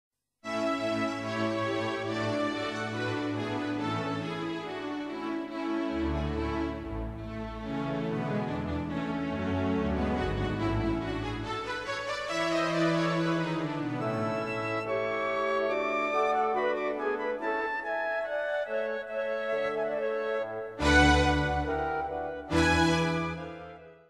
Eggs and Mozart, Salzburg
In Old Salzburg there is a shop selling nothing but decorated eggs.  My trusty iPhone videoed it and I added some Mozart from my copyright-free library.  Only a few seconds but I hope you enjoy it.